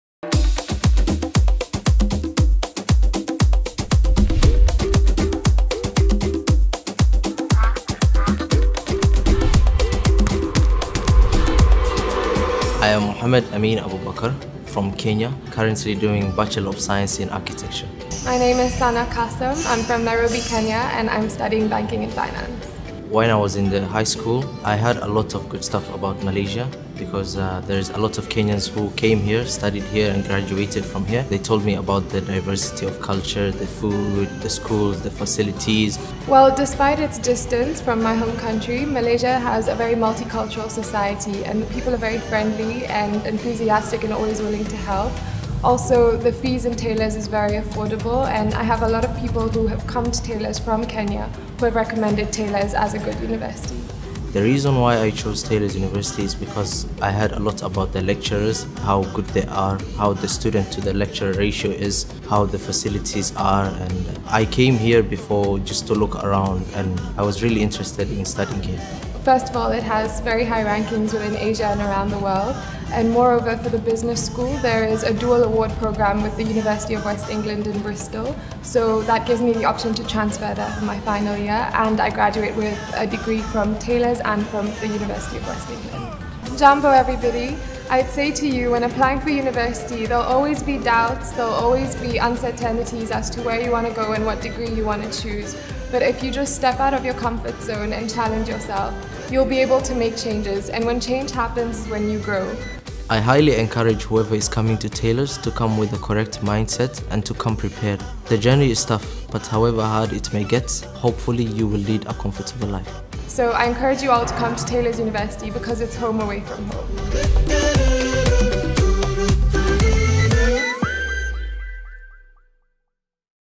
Life As A Taylorian – From Our African Student, Mohamed Amin Abubakar & Sanah Inayat Kassam
Find out why Taylor’s University, Malaysia is the preferred choice for our students from Africa. From world-class campus facilities to inspiring and helpful lecturers, our students share their experiences and how they adjust to a new environment abroad.